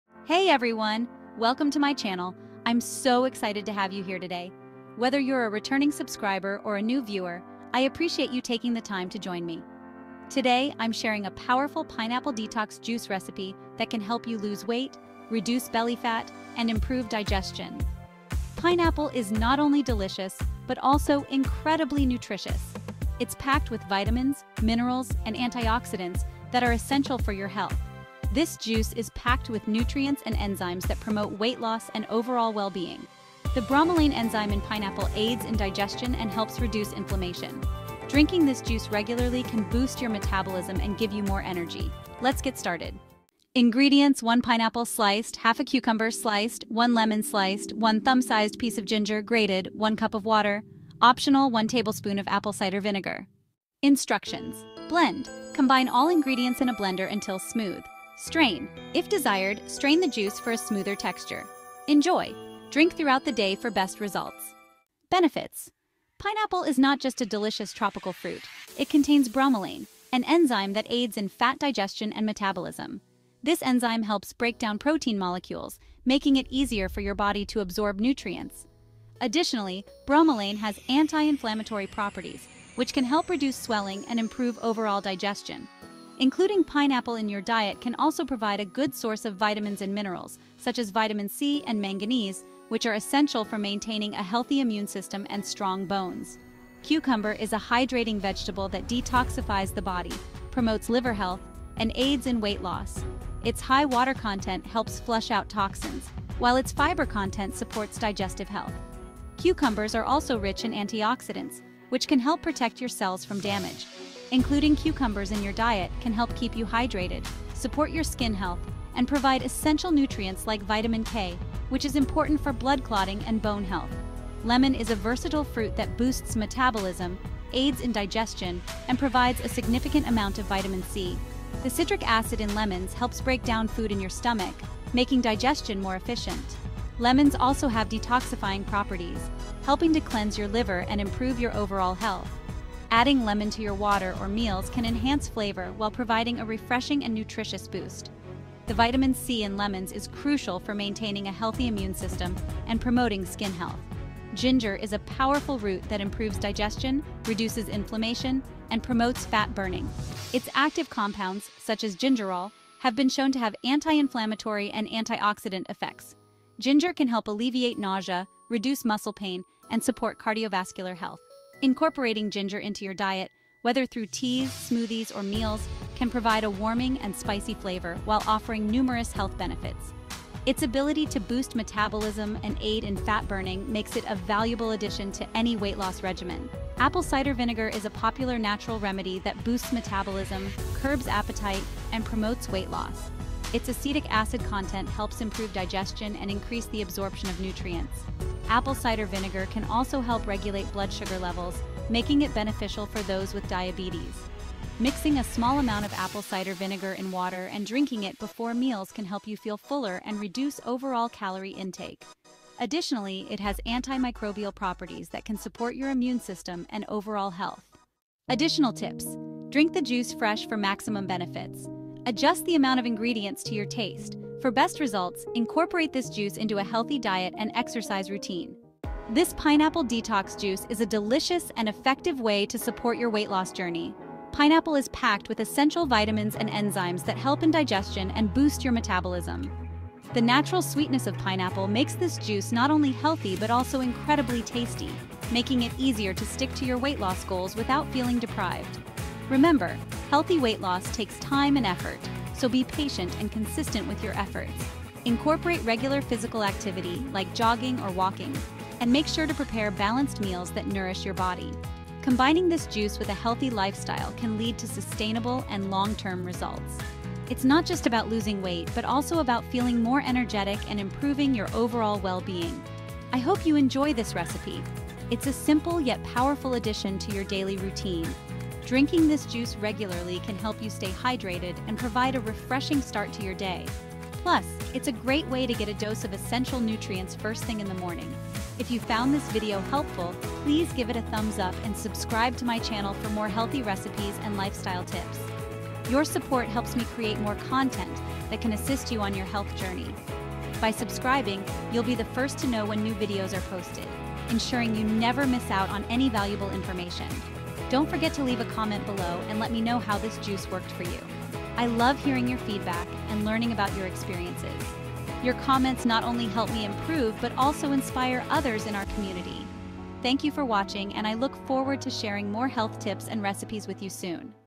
Pineapple Detox Miracle: Shed 30lbs Fast!
Looking for a natural and effective way to shed pounds quickly? In this video, I’ll show you how to make a powerful pineapple detox juice that can help you lose up to 30lbs fast! Pineapples are packed with vitamins and fat-burning properties, making them perfect for weight loss and detoxification. Watch as I break down the ingredients and steps to make this refreshing drink, plus I'll explain the amazing benefits of pineapple, ginger, and cucumber for your health. Whether you're on a weight loss journey or just looking to cleanse your body, this detox recipe is for you! Drink it daily to see results.